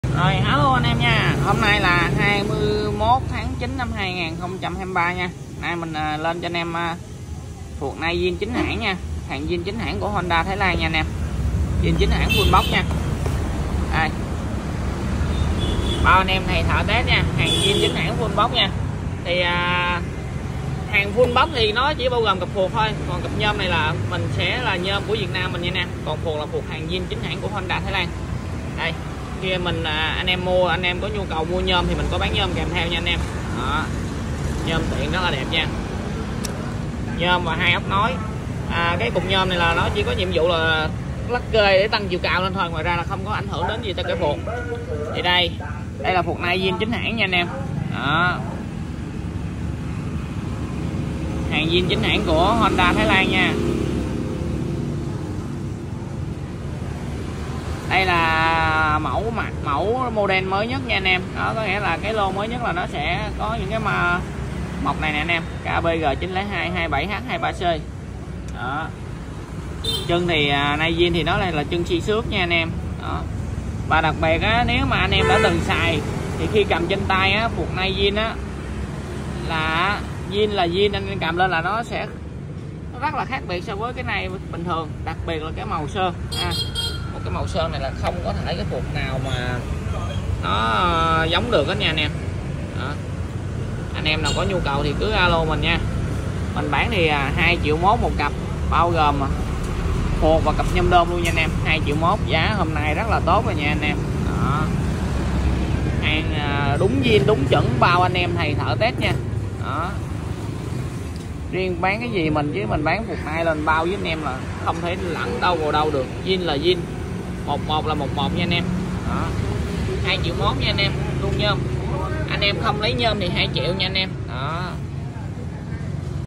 😍 2,1 Triệu Cặp Phuộc Nice Zin Mới 100 % chính hãng Honda Thái Lan 👉Giá: 2100K / Cặp( 21/9/2023 )
🚀 21/9/2023

✌ Phuộc Nice Zin Mới 100 % chính hãng Honda Thái Lan

👉 Giá: 2100K / Cặp ( Đã bao gồm cặp nhôm Đôn )

⛔️ Bao ae thầy thợ kiểm tra 👌

👉 Chiều cao phuộc phù hợp với nhìu dòng xe sử dụng từ Honda Yamaha SYM như Wave dream cub 67 Alegent ...

👉 Hàng Zin nên Bao nhún nhảy cực ngon

👉 Rất đầm khi đi xe chạy ở tốc độ cao

+ Agribank, CN Bình Tân, HCM
+ Đông Á Bank, CN Trường Chinh , HCM
+ Sacombank , CN Hốc Môn, HCM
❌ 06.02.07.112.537